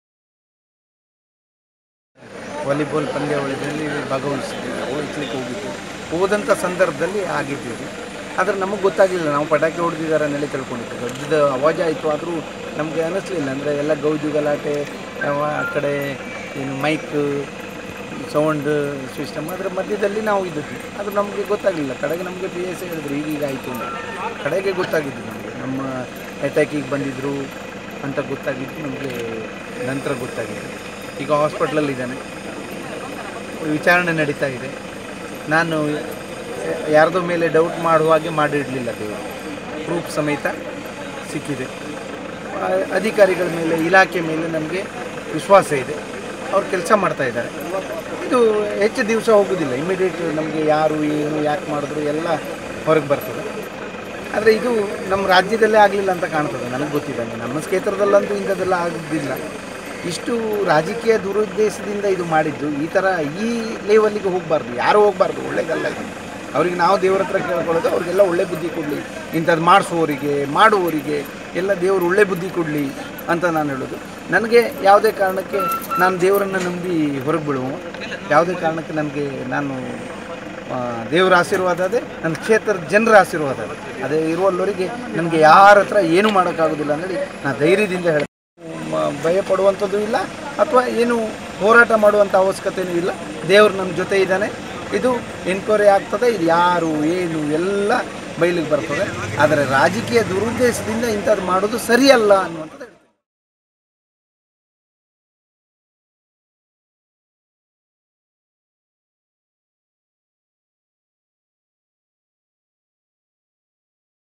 MLA Mankal Vaidya speaks with Media, reacts to the crude bomb attack on him in Honnavar
Bhatkal MLA Mankal Vaidya speaks with Media, reacts to the crude bomb attack on him in Honnavar